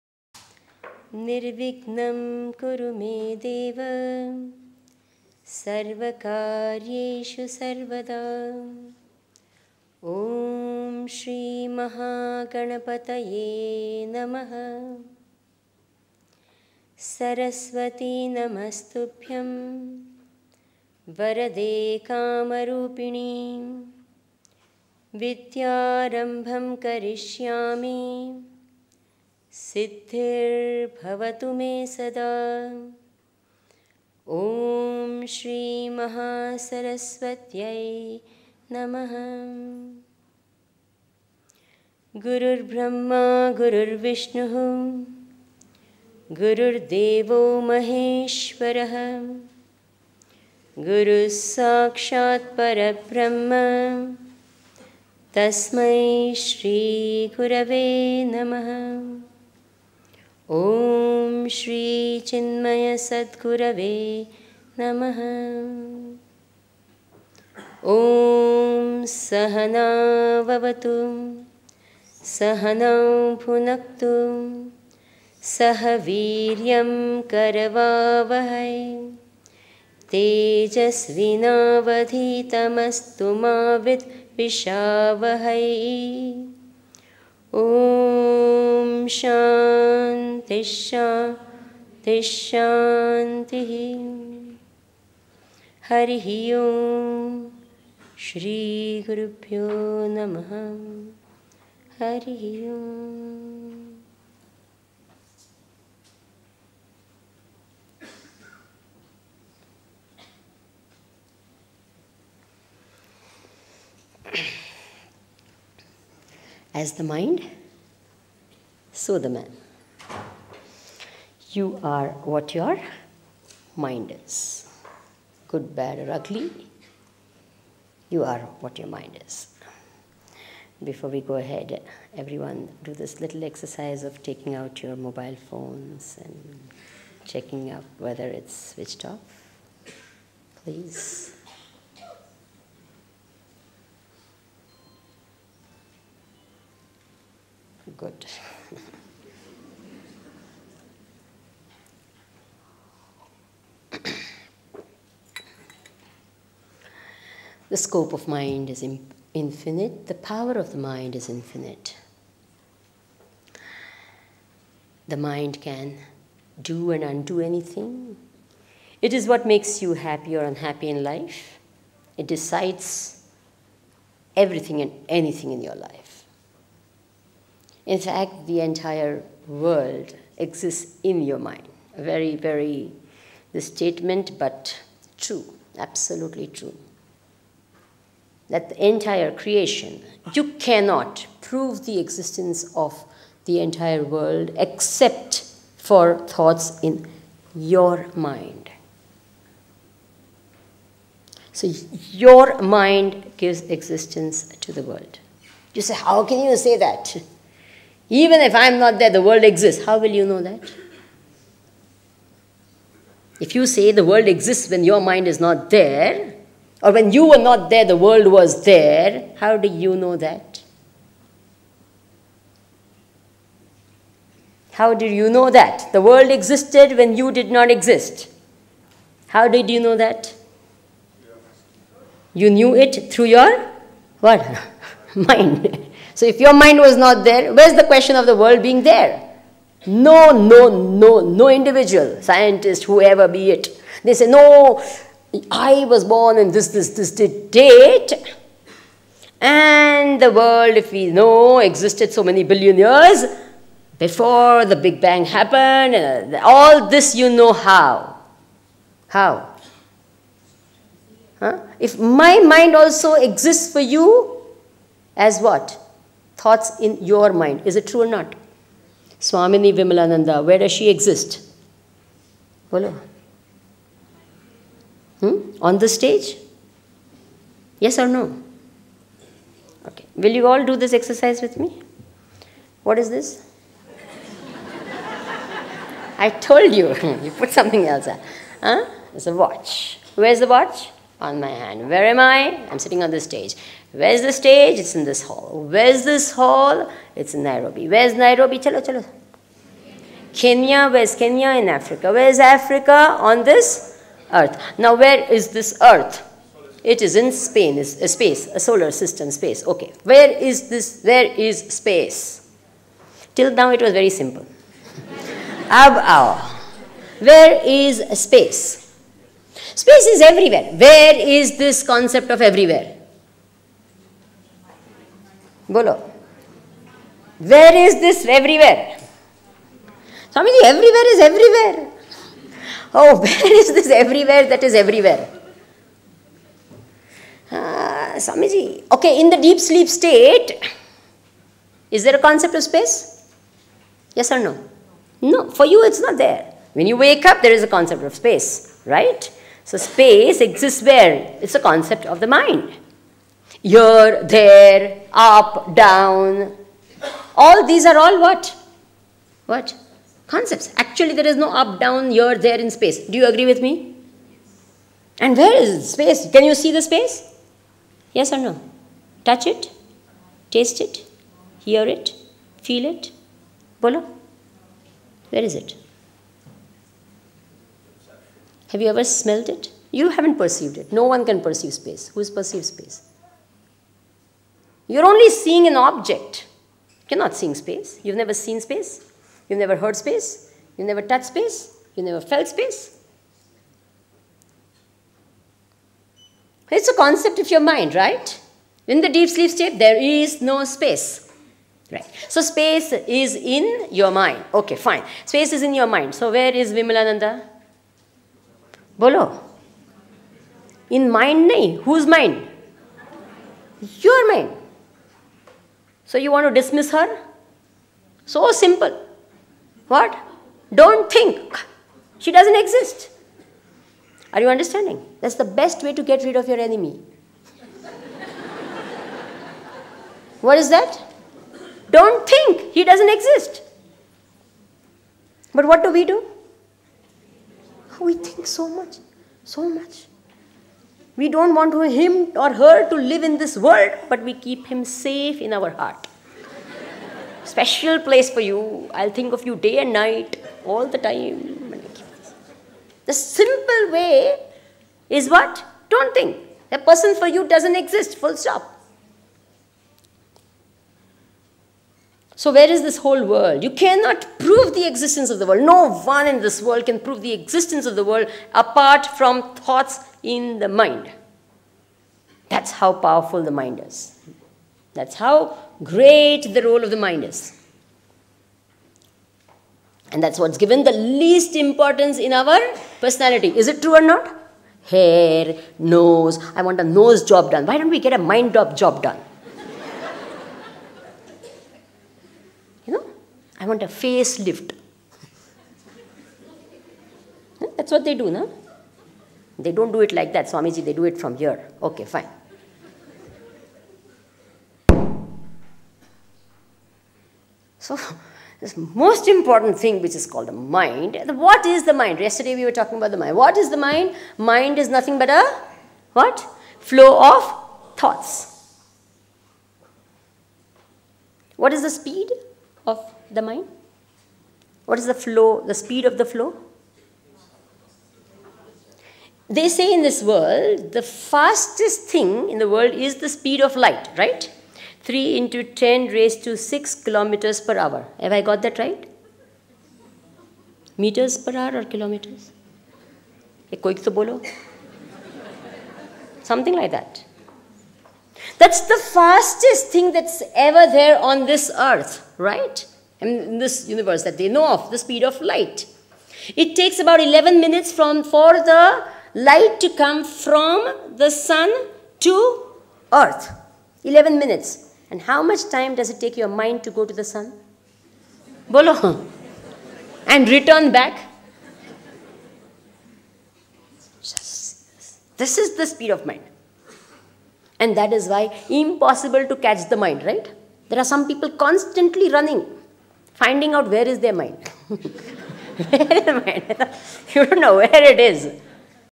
Understanding Emotions by Swamini Vimalananda Part 1
Based on lecture given by Swamini Vimalananda from Chinmaya Mission in Nairobi